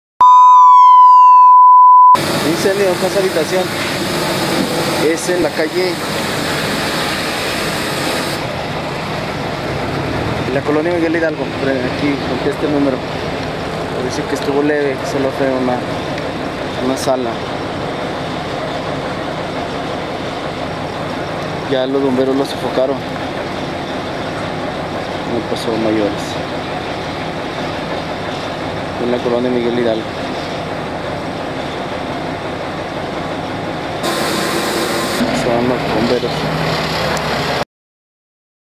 Se incendia casa
Se suscita un incendio en vivienda particular perteneciente a la Colonia Miguel Hidalgo.